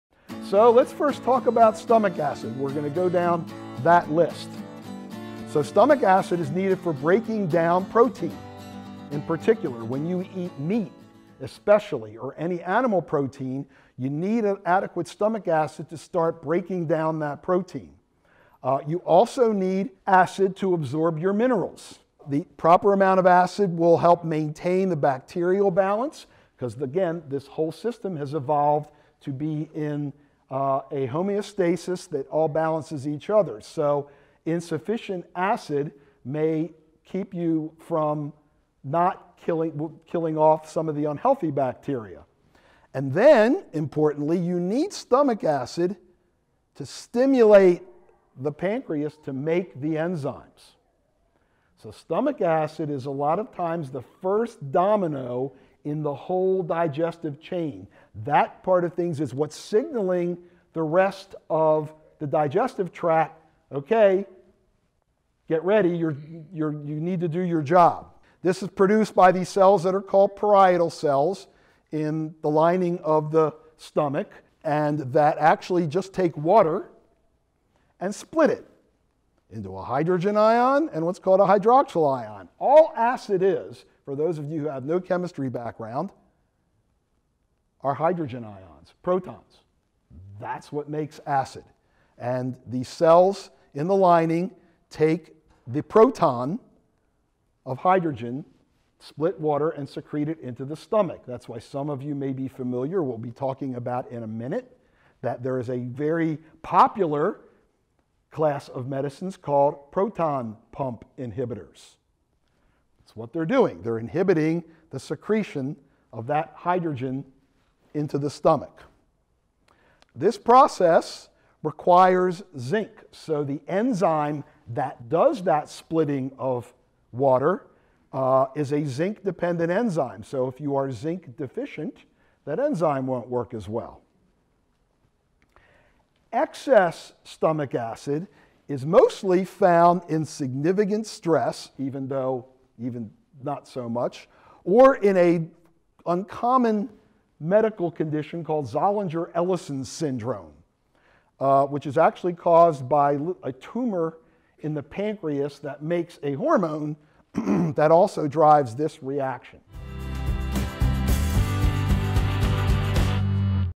A Healthy Gut Begins with Stomach Acid - Complete Care Founder: Dr. Thaddeus Gala
A healthy gut begins with adequate levels of stomach acid.  Stomach acid starts the domino effect of the whole digestive chain, and it is responsible for maintaining a healthy bacterial balance.